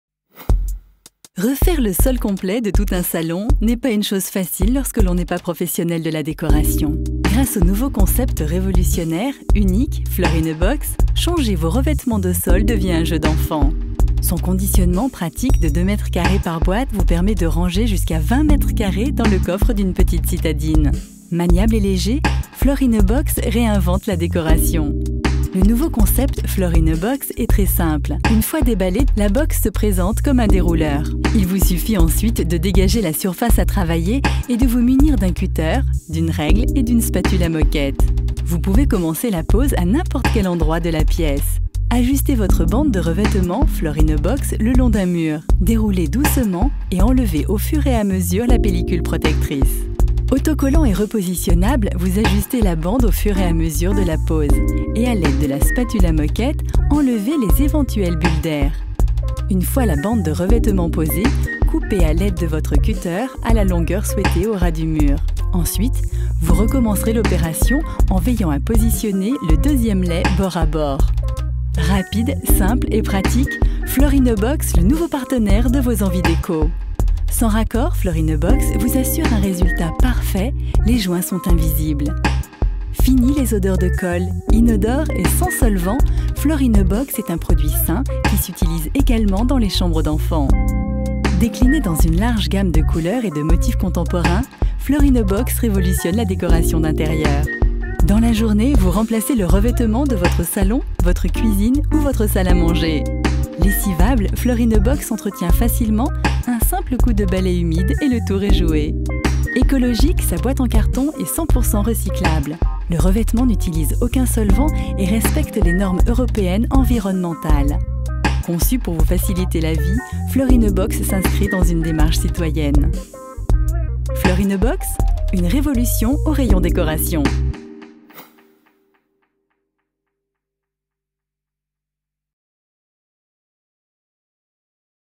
Floor in a box un revêtement de sol pvc en rouleau adhésif
Rapide et facile à poser le revêtement vinyle floor in a box est idéal pour changer la décoration de votre pièce en un clin d'œil